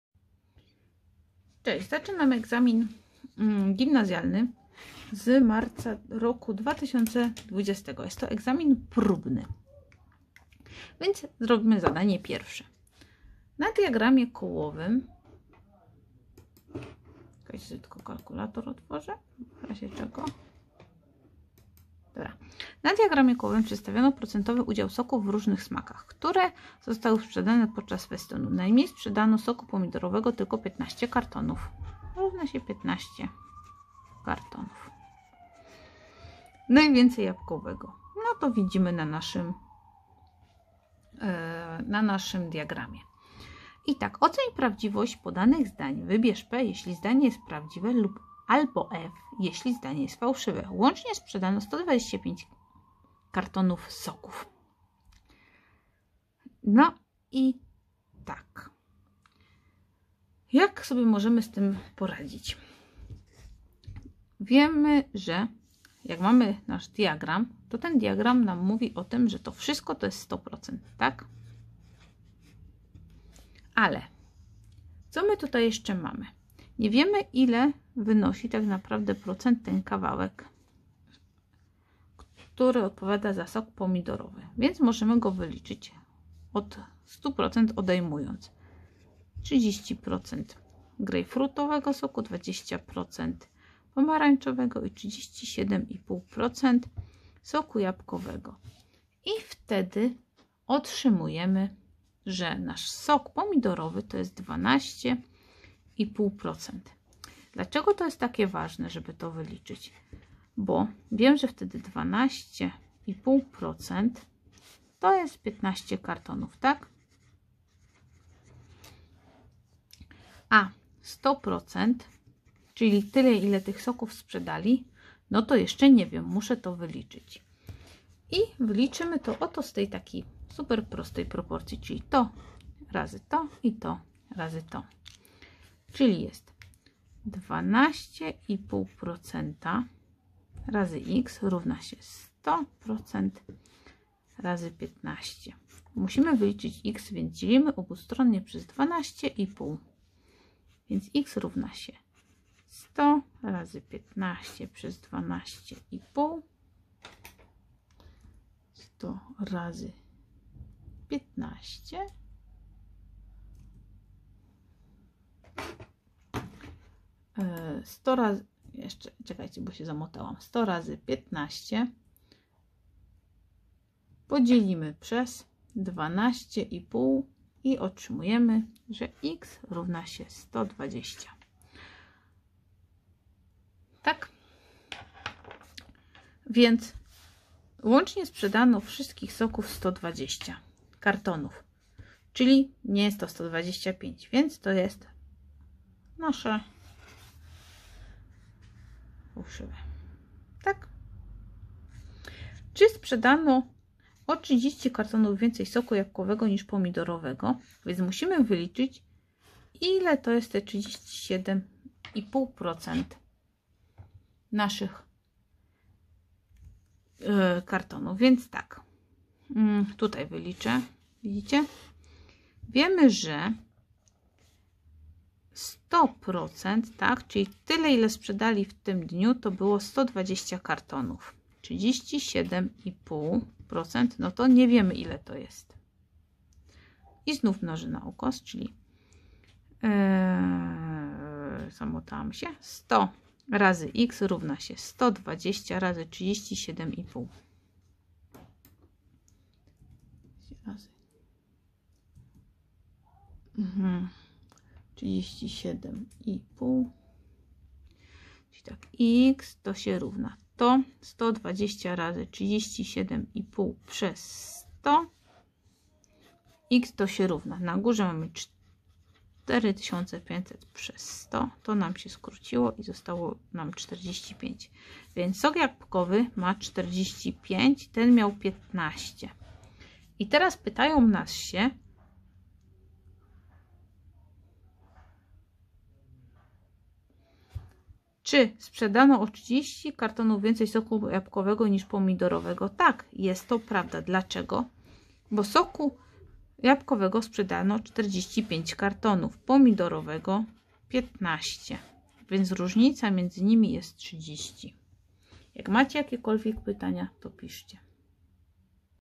Na diagramie kołowym przedstawiono procentowy udział soków o różnych smakach, które zostały sprzedan
Zadanie 1.
Na diagramie kołowym przedstawiono procentowy udział soków o różnych smakach, które zostały sprzedane podczas festynu. Najmniej sprzedano soku pomidorowego, tylko 15 kartonów, a najwięcej - soku jabłkowego.

Oceń prawdziwość podanych zdań. Wybierz P, jeśli zdanie jest prawdziwe, albo F - jeśli jest fałszywe.

Sprzedano łącznie 125 kartonów soków.
Prawda
Fałsz
Sprzedano o 30 kartonów więcej soku jabłkowego niż pomidorowego.
Prawda
Fałsz